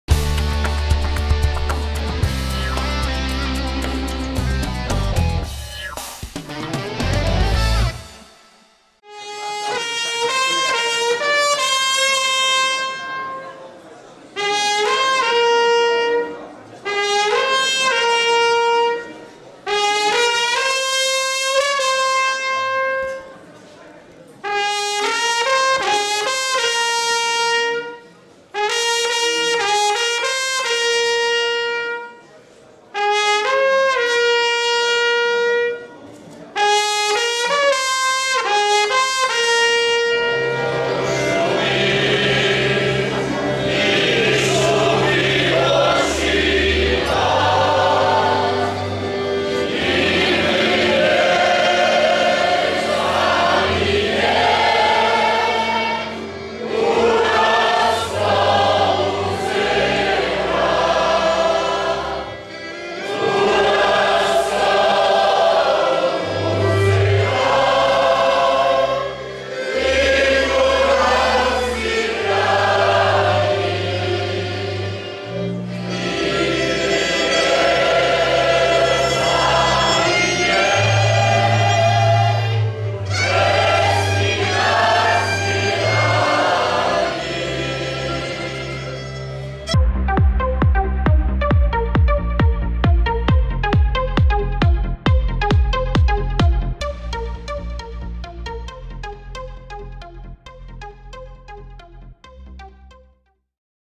[FLESZ] Obchody jubileuszu Istebnej i Jaworzynki wystawa w GOK
Obchody jubileuszy Istebnej i Jaworzynki oraz
 Rok Górali w Województwie Śląskim na terenie  Trójwsi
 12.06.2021 wystawa w GOK Istebna